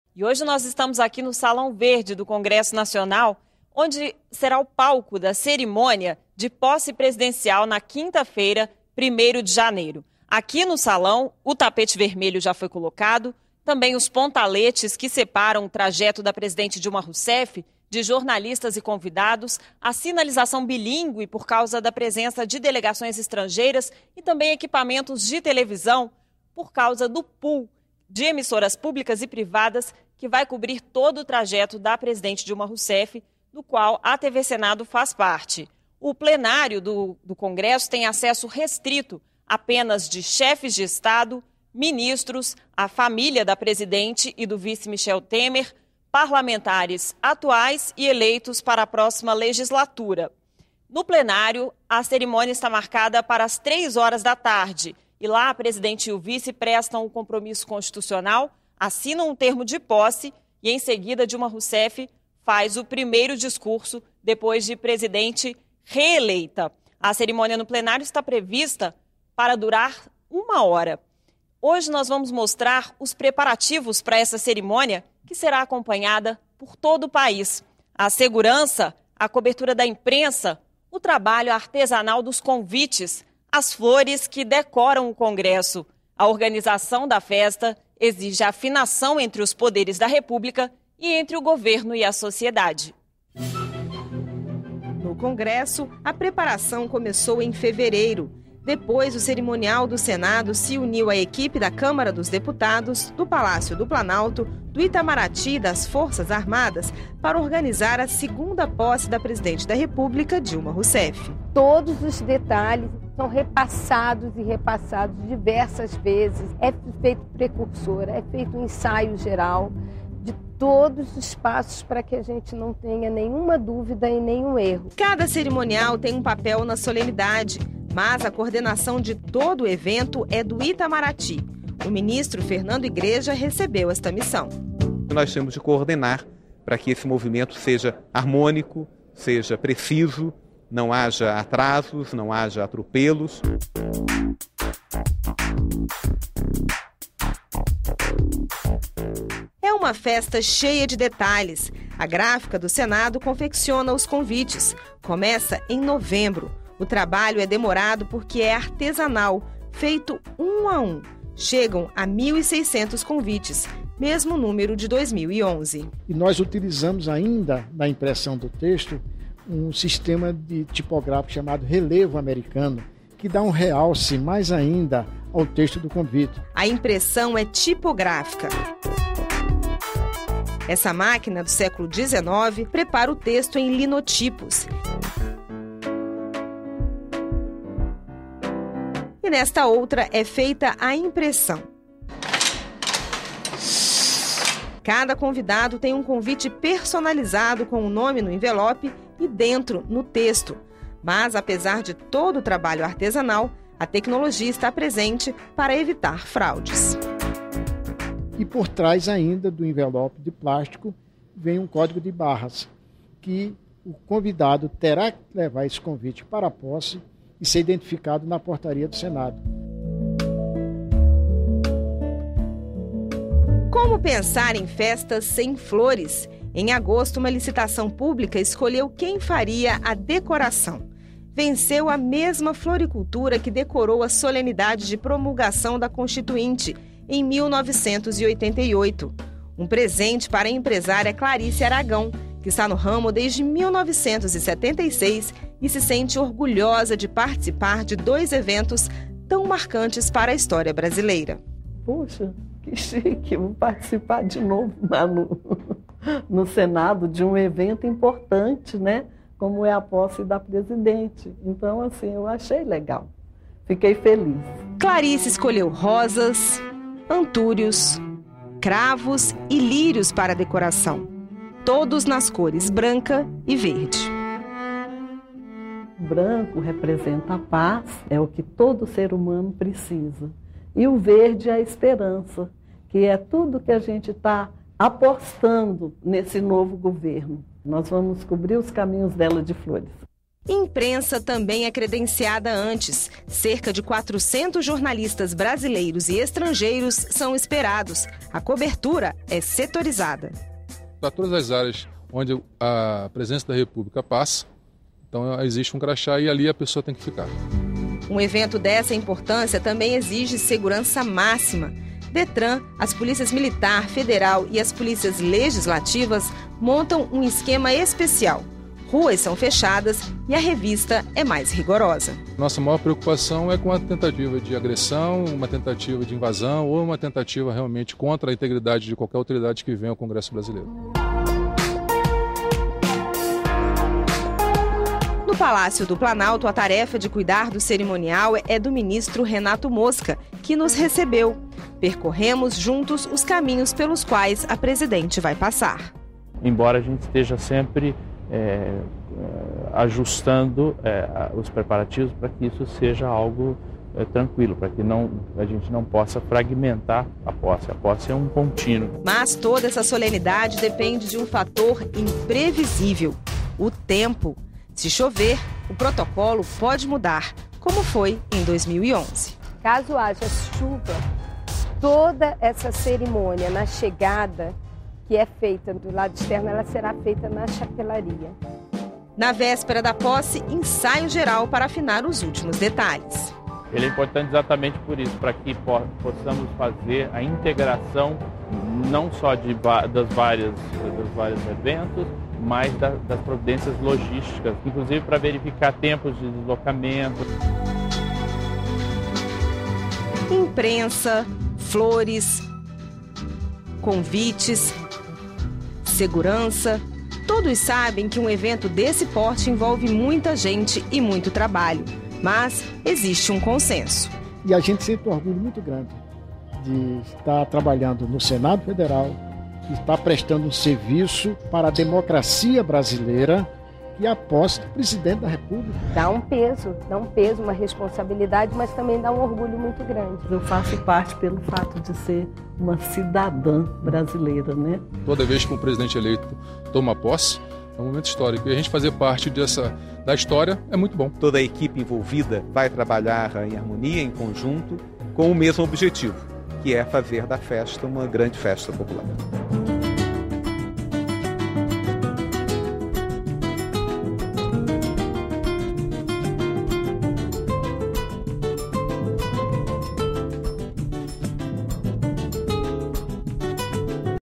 Jornalismo - A organização da solenidade de posse presidencial
A segurança, a cobertura da imprensa, o trabalho artesanal dos convites, as flores que decoram o Congresso. A organização da festa exige afinação entre os poderes da República e entre o governo e a sociedade.